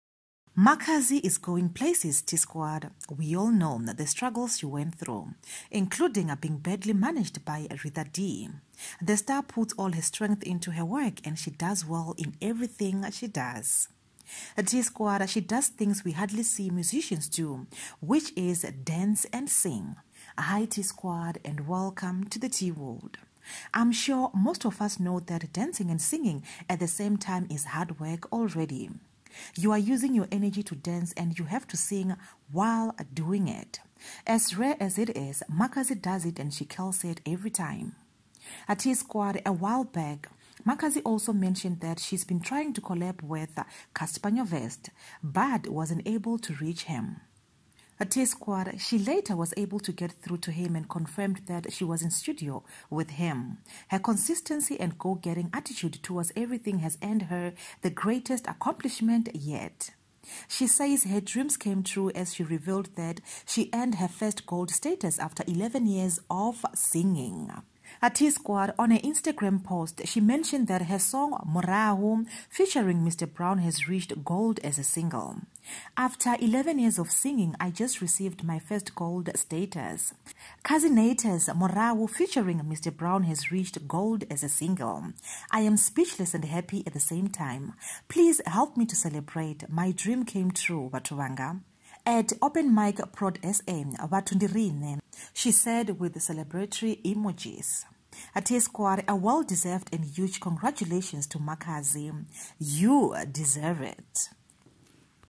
WOW Congratulations Makhadzi
Makhadzi is going places t squad. We all know the struggles she went through, including being badly managed by Rita Dee. The star puts all her strength into her work and she does well in everything she does. Tea squad she does things we hardly see musicians do which is dance and sing.

I'm sure most of us know that dancing and singing at the same time is hard work already. You're using energy to dance and you have to sing while doing it. As rare as it is, Makhadzi does it and she kills it everytime.